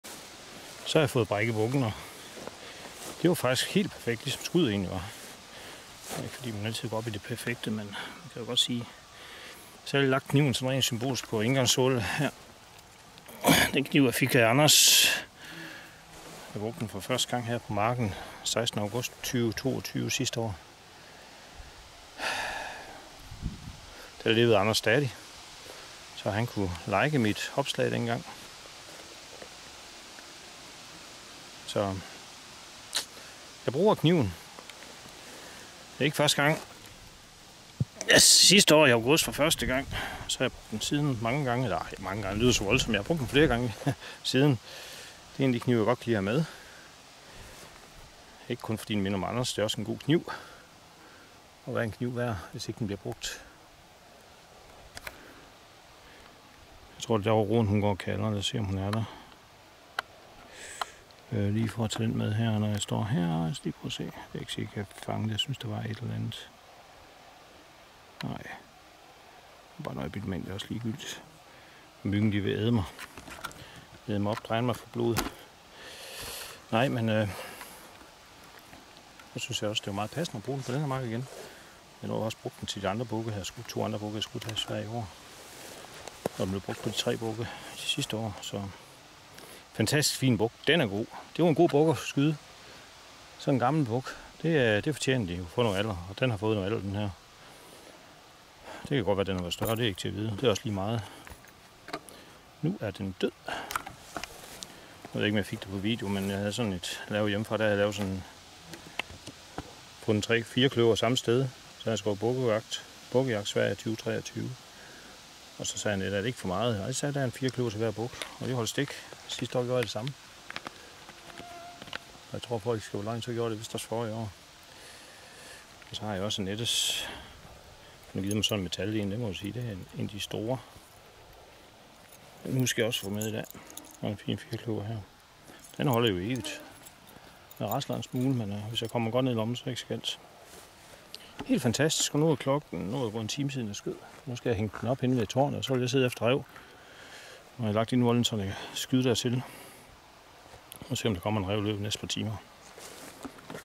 Bukkejagt/Bockjakt Sverige 2023 hvis man er vågen, så står der en buk og kigger på mig..
Det var Annette der opdagede det. På denne film, mens jeg står og har brækket min buk, filmer jeg rundt, og som et spøgelse dukker der et kort øjeblik en vågen buk op på filmen..men den er der kun kort tid..jeg ser den ikke, kameraet fanger den..